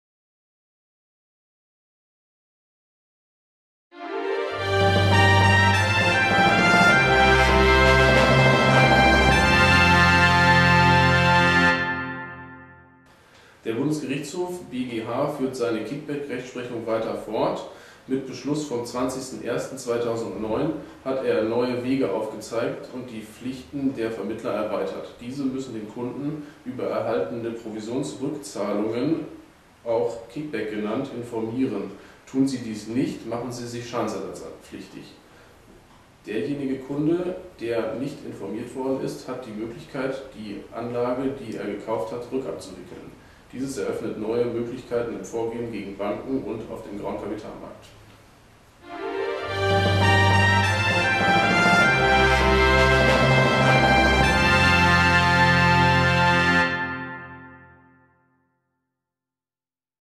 Advoclip Dr. Schulte: BGH Beschluss zu "Kick Backs"
Rechtsanwalt Sven Tintemann erklärt die Auswirkung der neuen Rechtsprechung des Bundesgerichtshofs zu Provisionsrückvergütungen (Kick Backs).